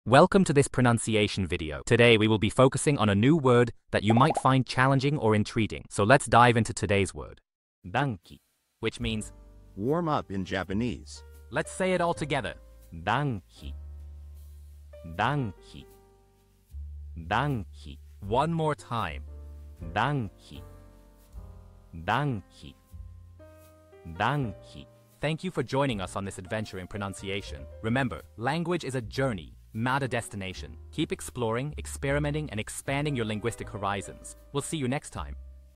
How to pronounce danki | 暖機 (Warm -up in Japanese)
Master the Pronunciation of 'danki | 暖機' - which means : Warm -up in Japanese 📝 with @PronunciationJapanese 🎓🗣️ - [Your Guide to Japanese]

Learn to pronounce 'danki' perfectly in Japanese with our easy and engaging tutorial by @PronunciationJapanese! 📖📽️ In this comprehensive video, we provide you with native speaker examples 🗣️👂 and phonetic breakdowns 🎵 to ensure you get the pronunciation just right.